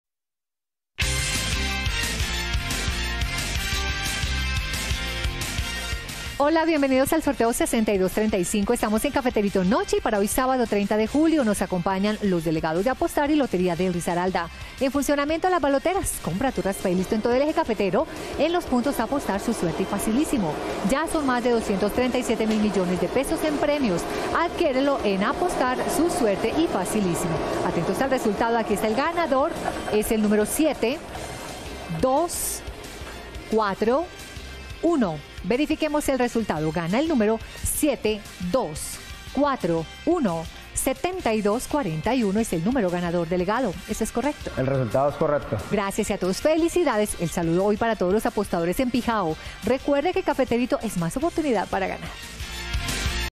CAFETERITO NOCHE Resultado del SÁBADO 30 DE JULIO de 2022 ✅🥇🔥💰
Hola buenas noches, te presentamos los resultados de las loterías y chances en Colombia, vamos con el Resultados del Chance El Cafeterito noche del SÁBADO 30 DE JULIO de 2022.

Resultado Extra de Colombia SÁBADO 30 DE JULIO de 2022